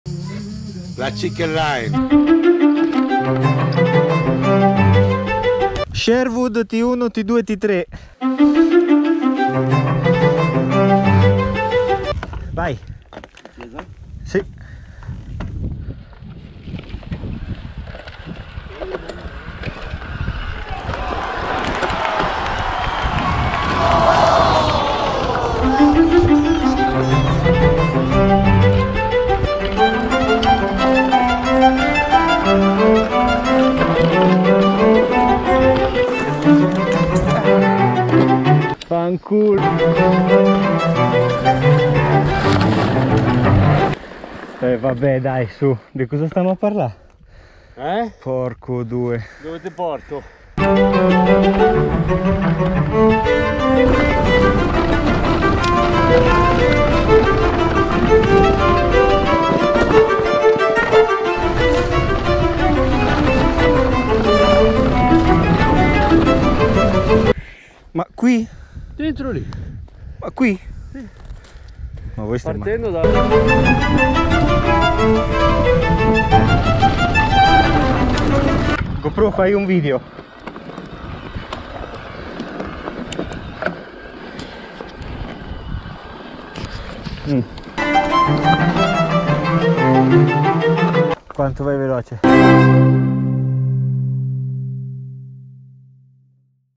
Come si mette la GoPro in mtb? Tutorial poco serio
Ritorniamo dopo qualche mese pronti per una stagione con mille novità.
Per cominciare oggi beccatevi questo video che spiega come (NON) indossare una GoPro in sella ad una mtb, girato fra le splendide colline tra San Severino Marche e Serrapetrona.